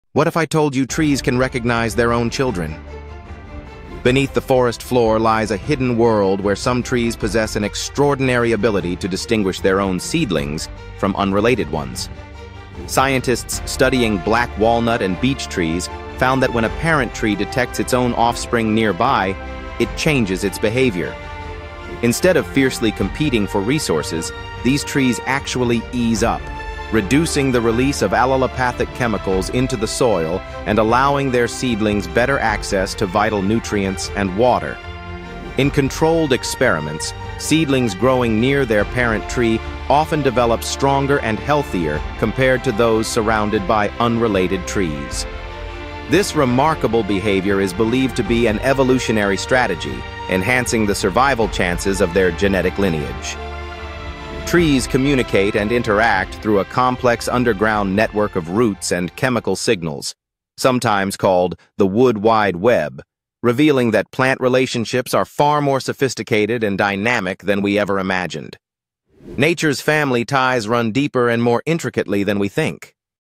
Can Trees Really Recognize Their Own Kids?
Did you know some trees can identify and nurture their own seedlings? Discover the mind-blowing science of plant family ties!